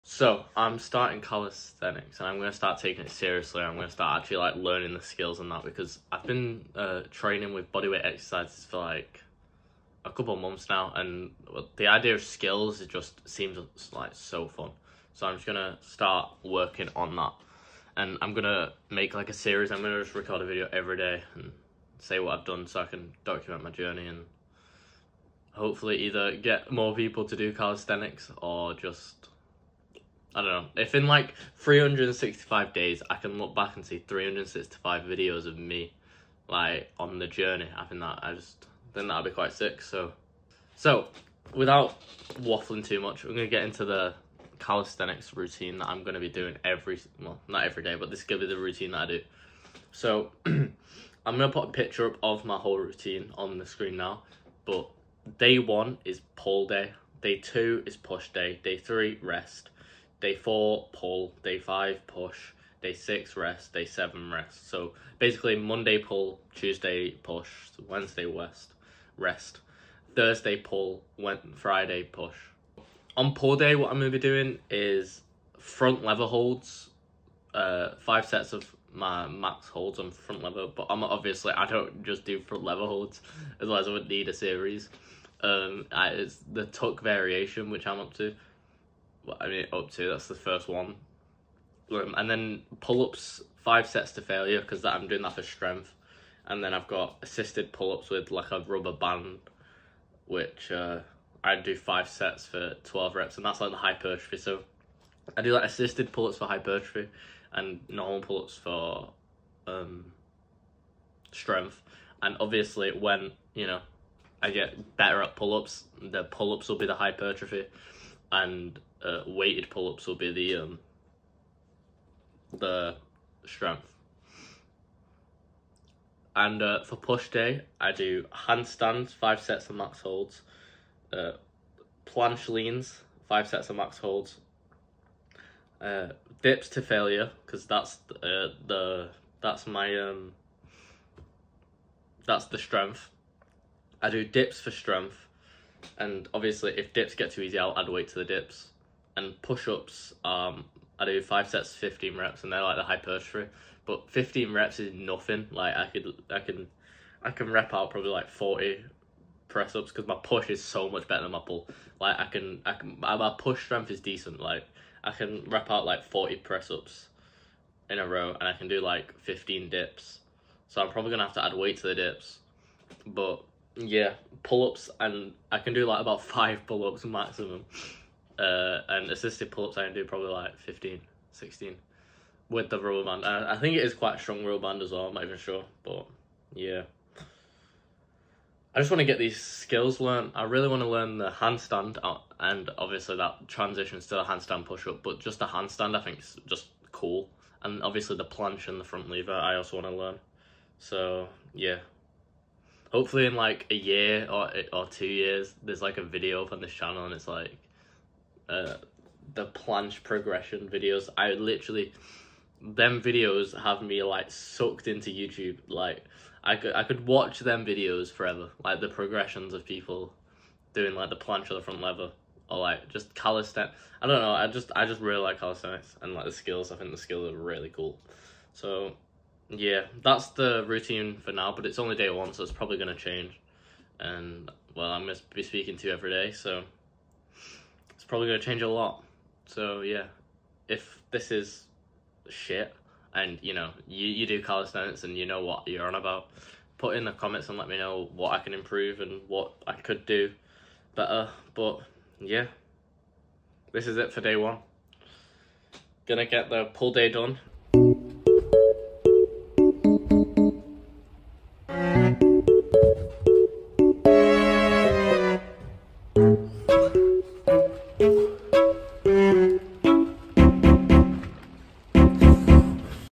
Calisthenics - day 1 (Start of the journey)
Today is the first day of my calisthenics journey, in this video I talk about my Training routine Goals and what is in store for the future. I am currently training for Front lever, planche  and handstand, ad on a PPL split

Also, put notifications on so you can stay up to date with my latest content!